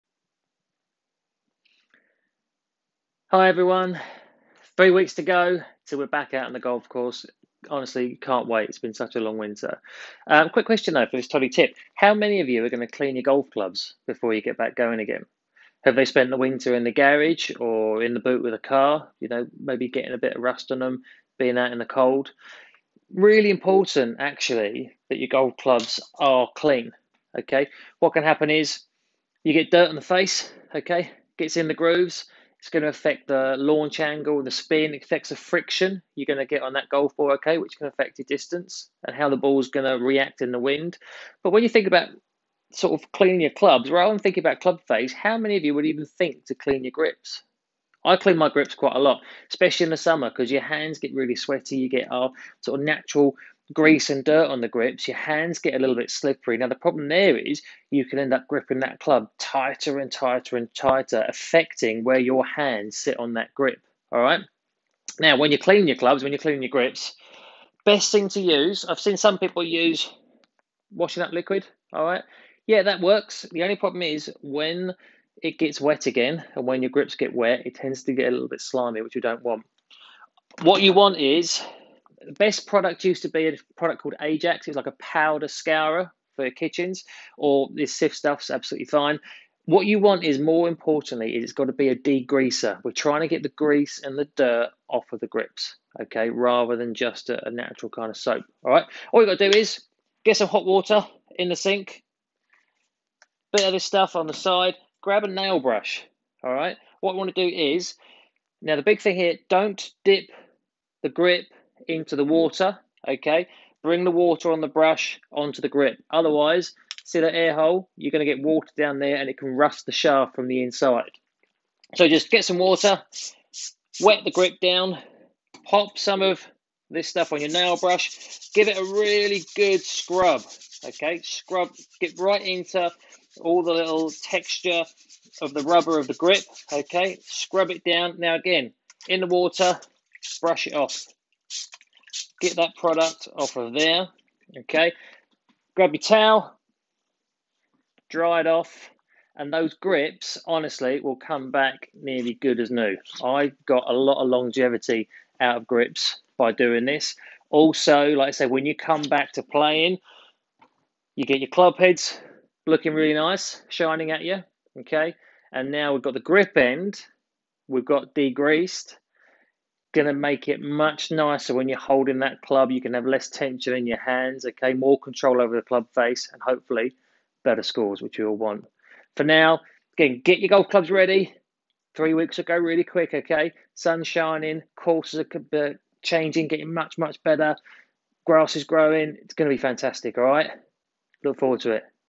How to clean your golf grips
When you clean your clubs do you also clean your grips? Here’s the best way to keep your grips feeling as good as new for longer.